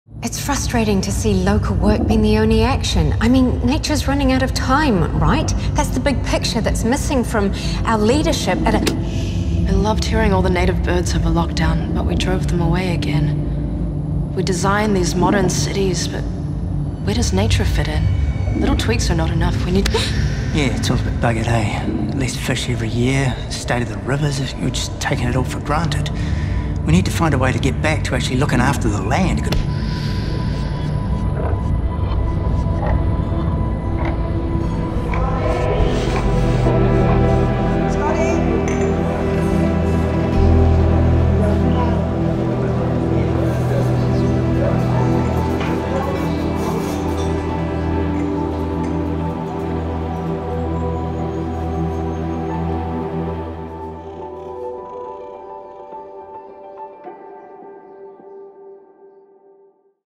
Let Yourself Speak For Nature
A recent study reveals more New Zealanders are worried about nature than we realise, yet they feel alone in their concerns and are afraid to speak up.

Directed by Alyx Duncan
Cinematography by Nina Wells
Edited by Adam-Luka Turjak